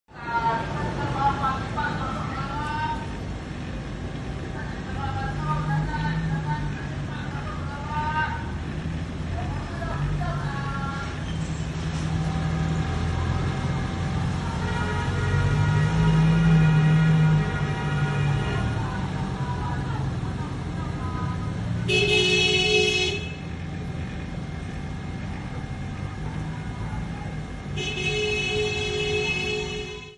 'Say No to PAS' Convoy in Kuching
A convoy of vehicles was seen in parts of Kuching, bearing posters urging Sarawakians to oppose and protest GPS' decision to support PN.